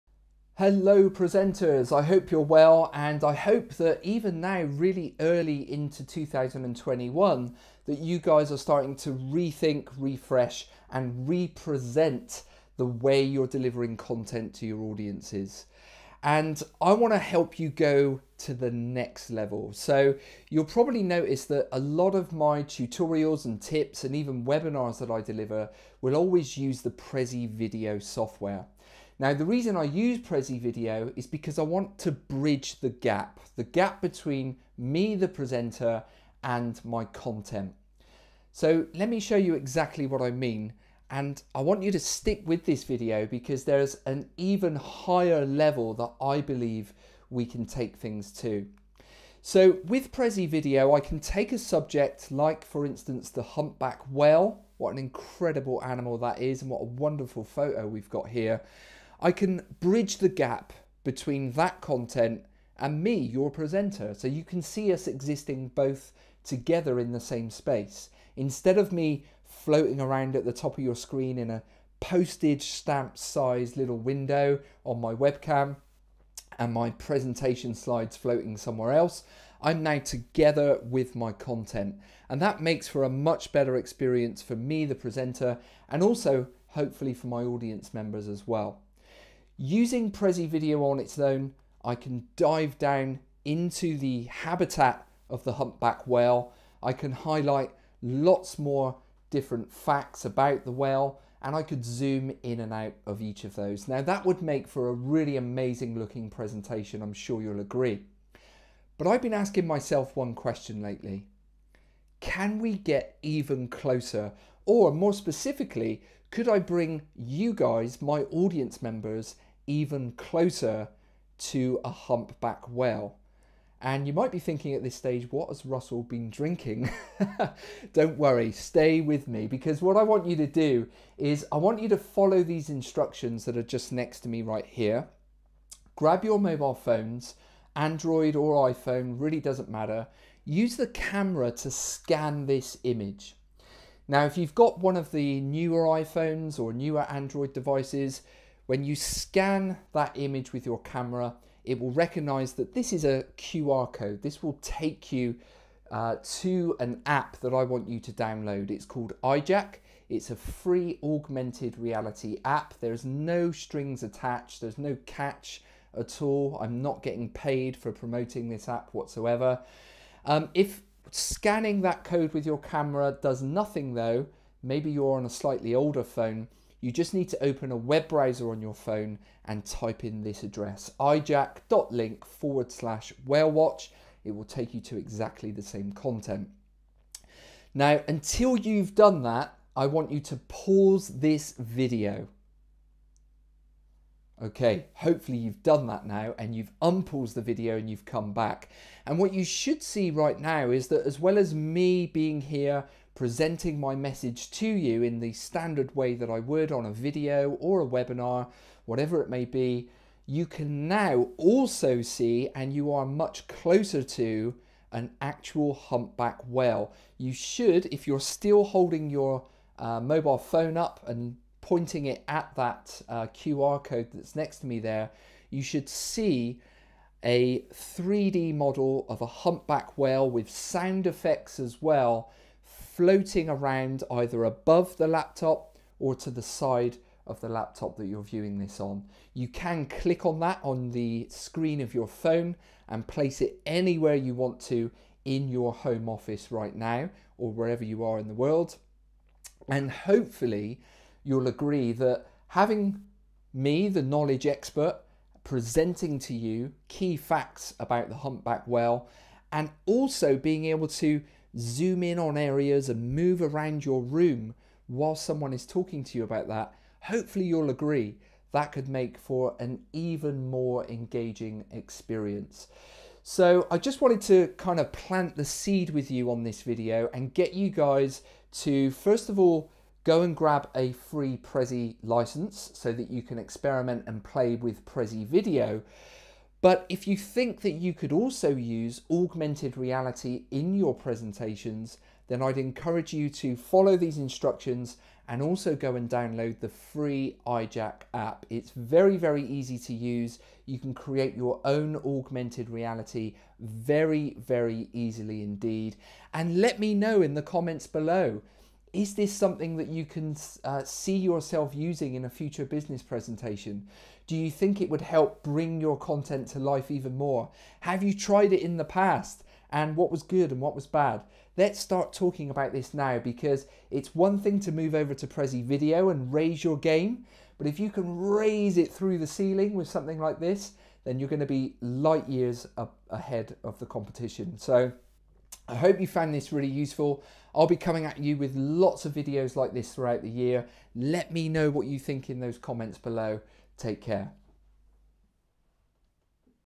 Prezi Video & Augmented Reality
Take your presentations to even greater heights by introducing a touch of augmented reality to them. Here's how you could do that with Prezi Video and the EyeJack app.